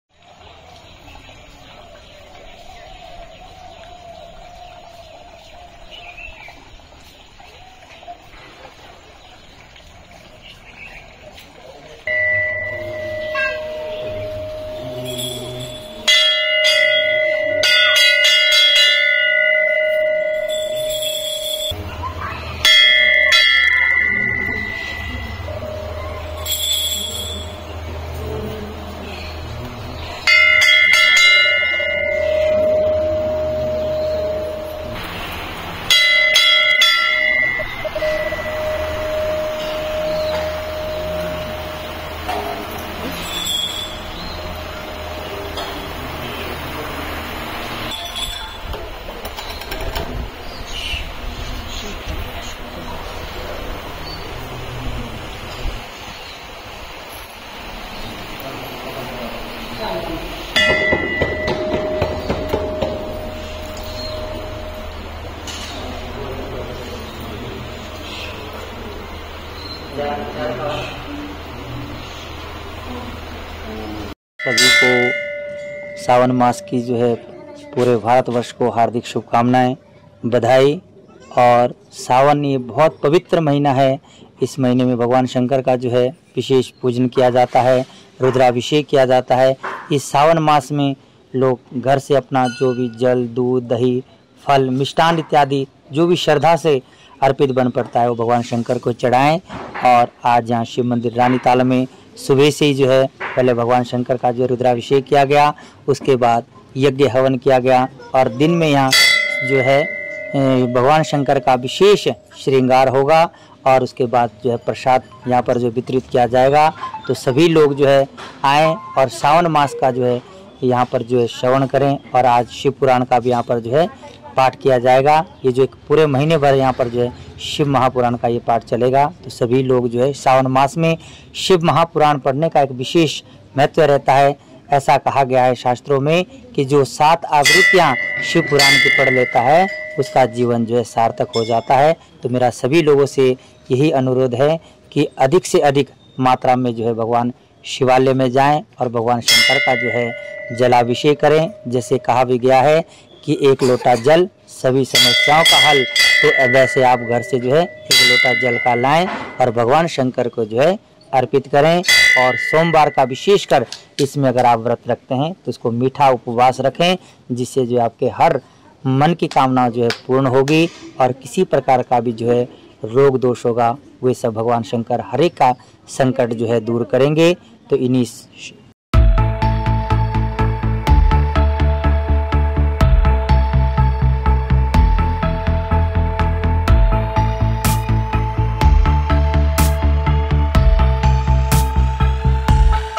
सावन का पहला सोमवार आज, शिव मंदिरों में उमड़ी श्रद्धालुओं की भीड़
, शिव मंदिर रानीताल में हुआ भगवान शिव के रुद्राभिषेक सावन मास का हिन्दू धर्म मे विशेष महत्व। एंकर : आज सावन मास का पहला सोमवार है और इस उपलक्ष्य पर शिव मंदिर में श्रद्धालुओं की सुबह से ही भीड़ देखने को मिल रही है। हिंदू धर्म में सावन मास सबसे पवित्र माना जाता है वहीं सोमवार के दिन इस महीने के दौरान शिव पूजा का विशेष महत्व है। वीओ 1 नाहन शहर के ऐतिहासिक शिव मंदिर रानीताल में सुबह 6:00 बजे से ही बड़ी संख्या में श्रद्धालु शिव मंदिर पहुंचे और यहां शिव पूजन किया। मंदिर में आज विशेष हवन यज्ञ कर सावन मास का शुभारंभ हुआ। मंदिर के पुजारी पंडित काकू राम शर्मा ने बताया कि सावन पवित्र महीना है और इस महीने में रुद्राभिषेक कर भगवान शिव का पूजन किया जाता है उन्होंने कहा कि इस पवित्र माह में लोग घर से जल ,दूध, दही लाकर शिव मंदिर में आकर अर्पित करें तो उनकी सभी मनोकामनाएं पूरी हो जाती है । उन्होंने कहा कि शिव मंदिर में आज भगवान शिव का रुद्राभिषेक किया गया और उसके बाद हवन यज्ञ का भी आयोजन किया गया। उन्होंने कहा कि आज भगवान शंकर का विशेष श्रृंगार मंदिर में किया जाएगा और मंदिर में आने वाले सभी श्रद्धालुओं को प्रसाद वितरित किया जाएगा। उन्होंने कहा कि शिव मंदिर रानीताल में पूरे महीने शिव पुराण पाठ का आयोजन किया जाएगा।